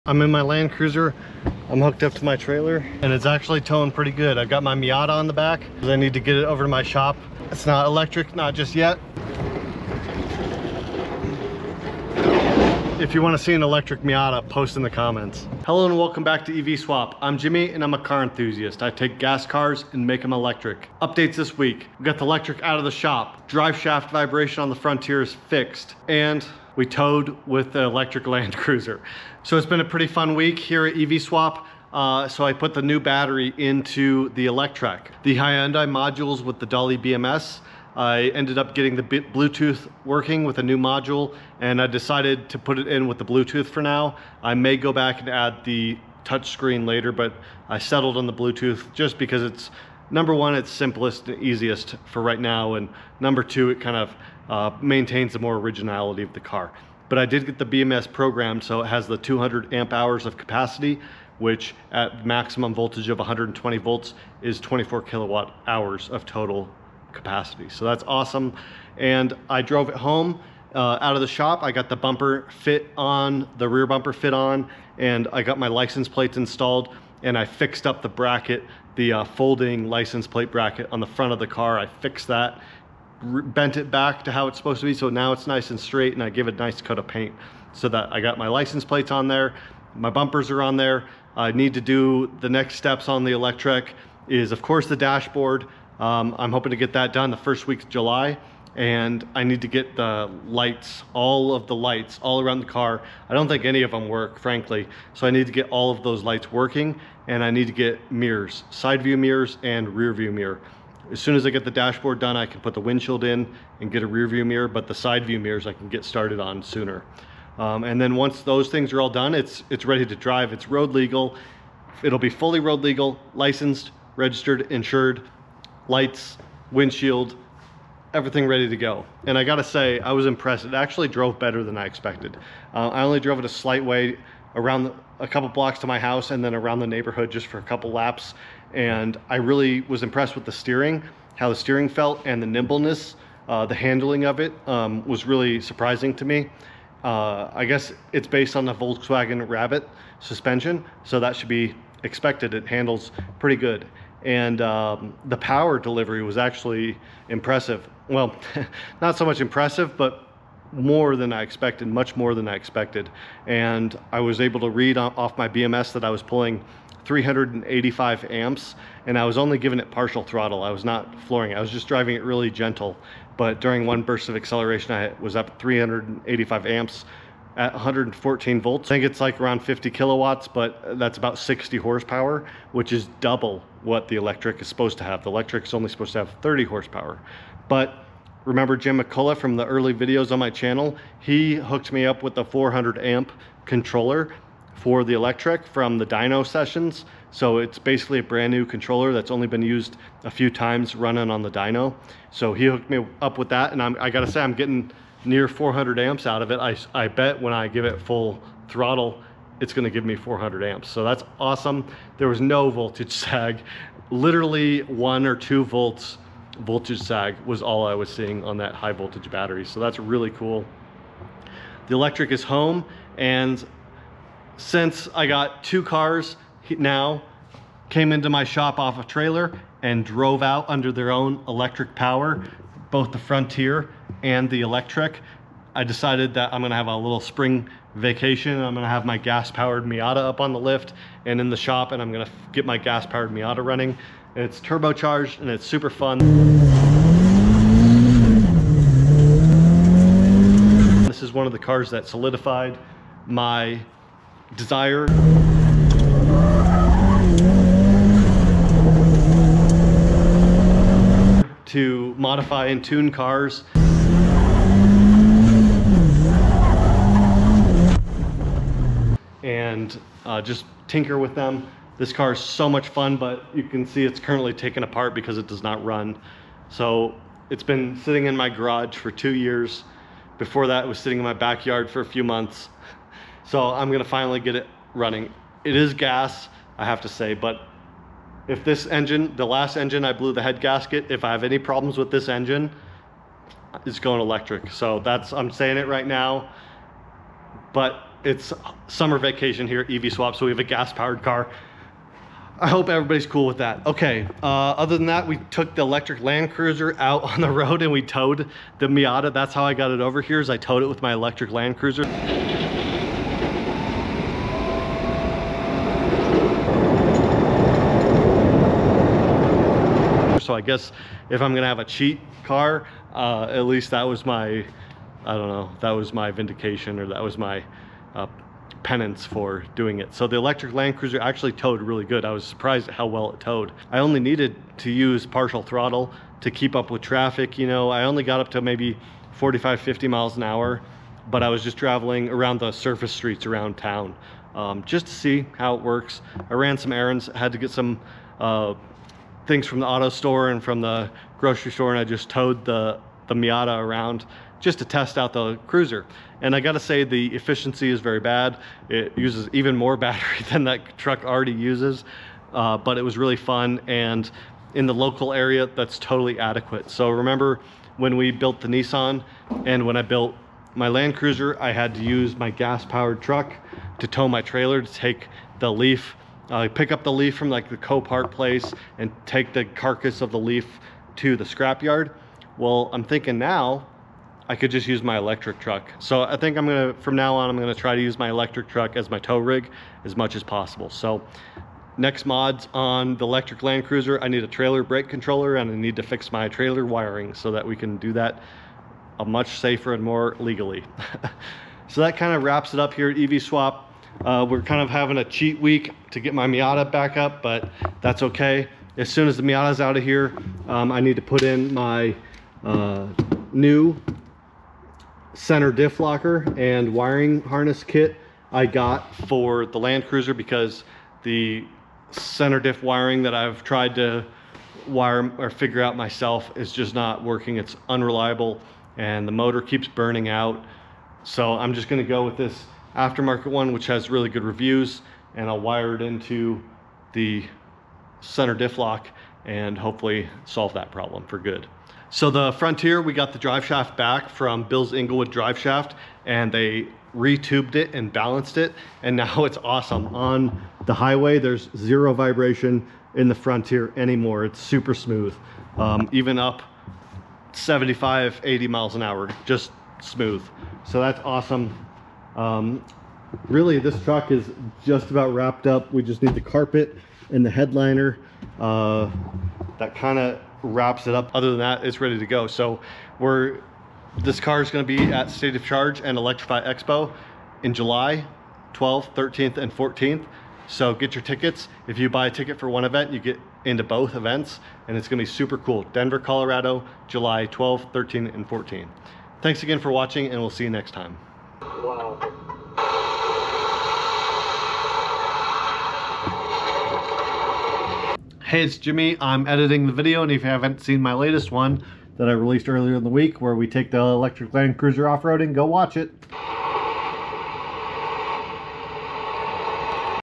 Miata, Electrek, Frontier, Land Cruiser...It's Summer Vacation at EVSwap
EV Miata? Electric tow rig? 40 year old fiberglass electric car restoration. What the heck is even going on at EVSwap?

All proceeds will go straight back into this YouTube Channel and our EV Builds!